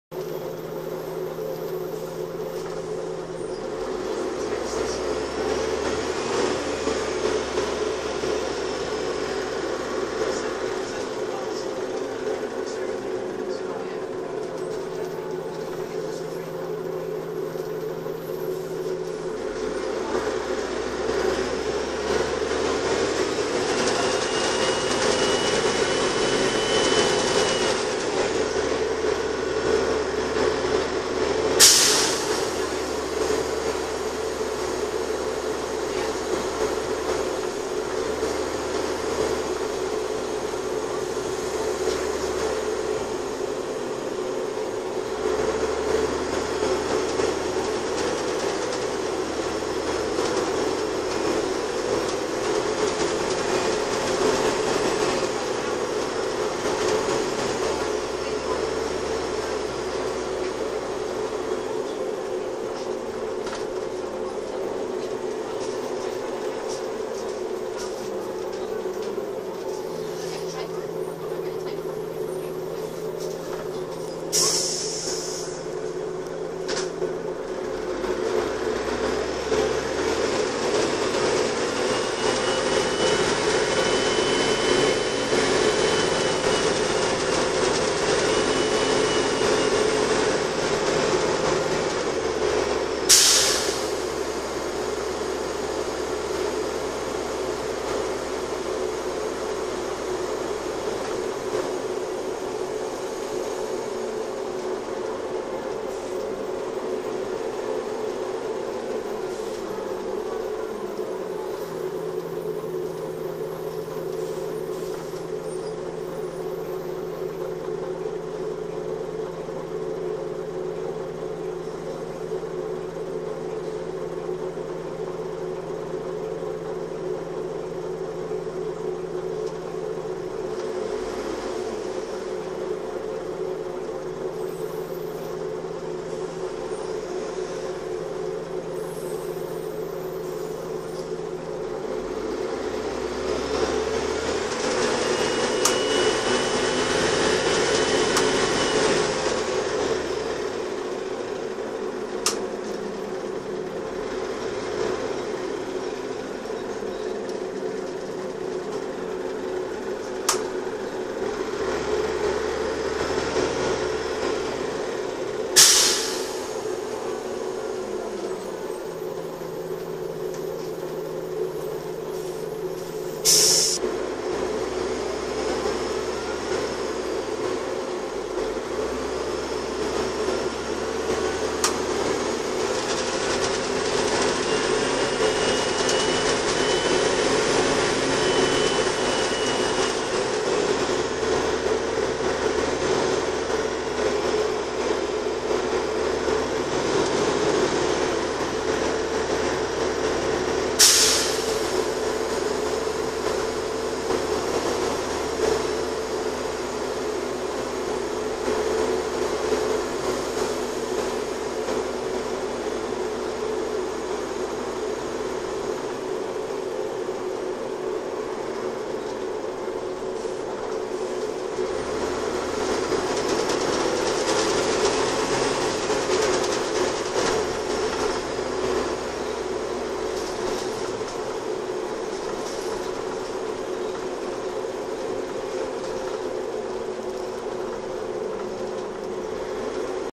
First Kernow 34049 P249 UCW
Here we see a journey on Volvo Olympian 34049 P249 UCW seen on route 17 to St Ives.